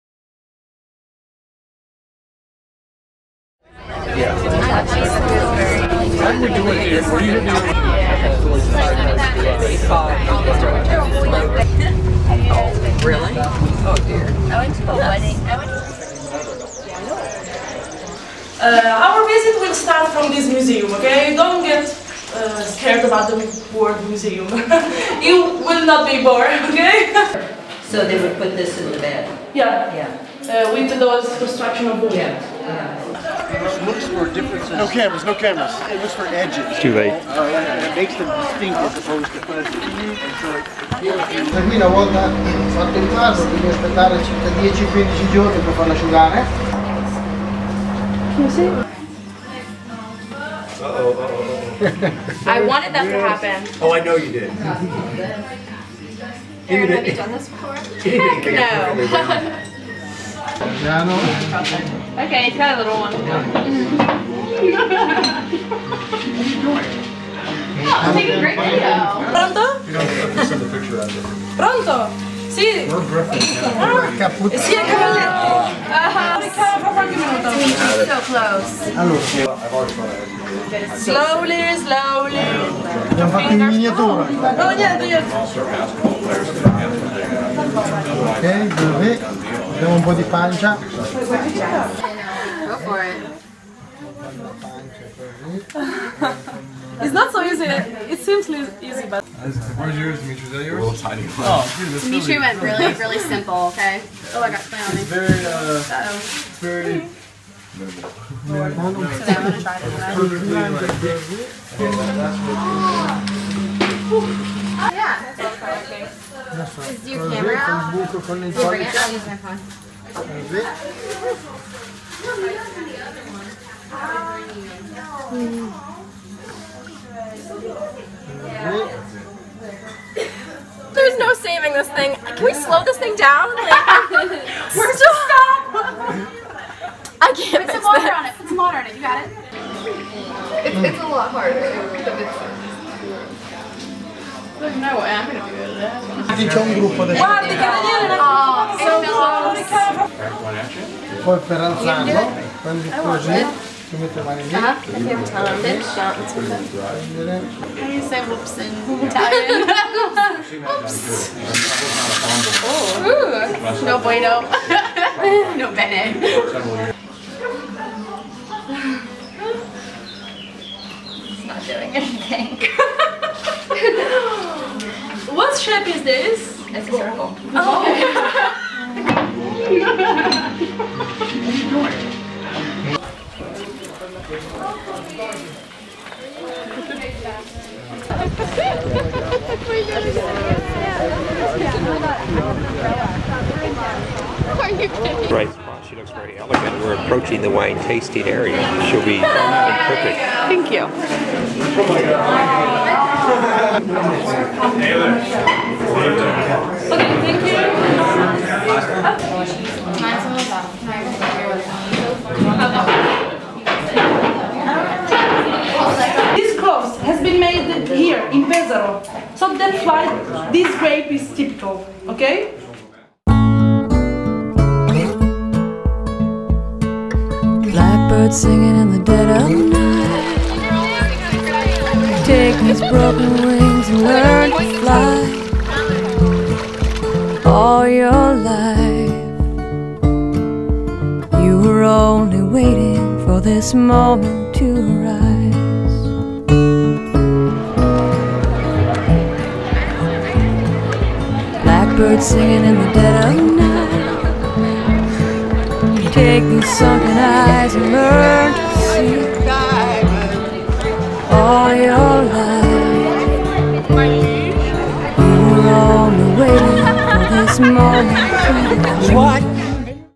Urbino Project- Pottery and Winery Trip
June 14, 2014.  Student and faculty fun field trip.